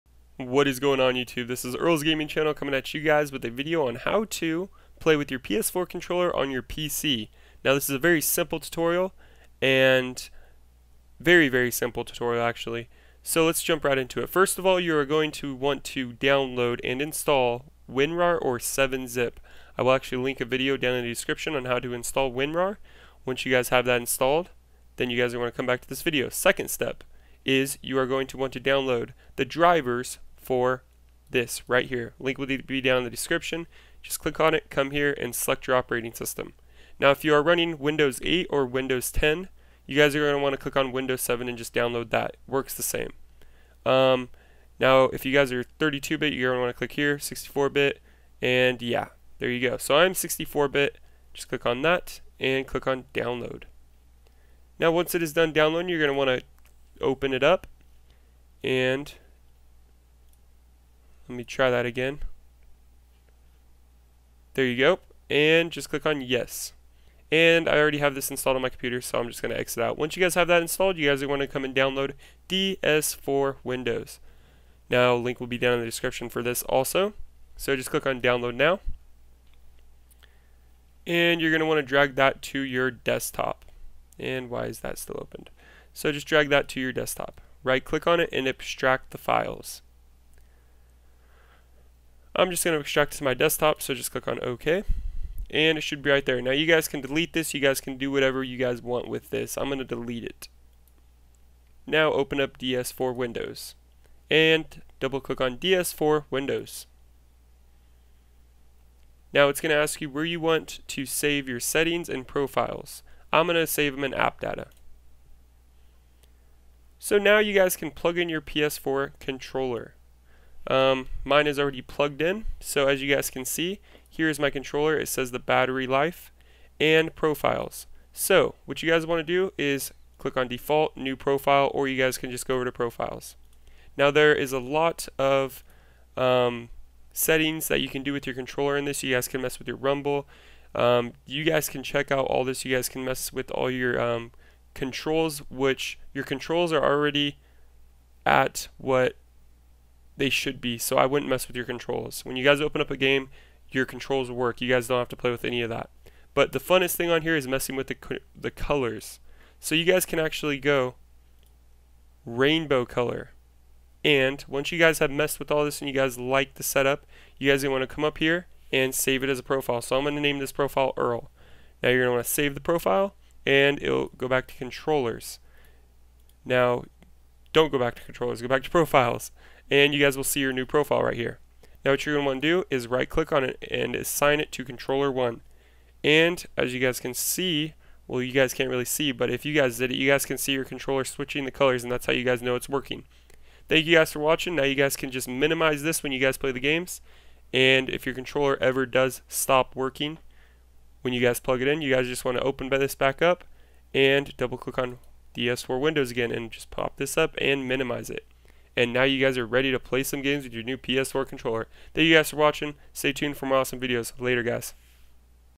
How to use PS4 controller on PC (Easy) -Best Way-
Hello youtube, today i am going to show you how to play games on your pc using your ps4 controller. This is the best way to do as you can change your light bar on your ps4 controller to any cooler! COOL RIGHT!? Please leave me feedback on my videos!

INSTRUCTIONS

Step 4: Open DS4Windows

Step 5: Plug in PS4 controller and your ready to play!

DOWNLOADS